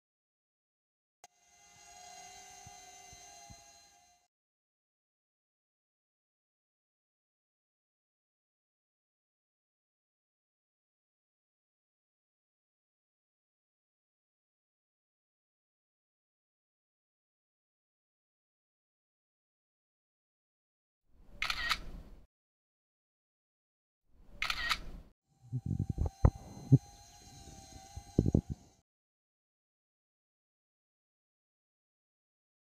Drone WK100 iPhone - vidéo / photos caméra embarquée
Test complet du drone WK100 de Wakera, pilotable par l'iPhone, à retrouver dans la rubrique tests de iPhon.fr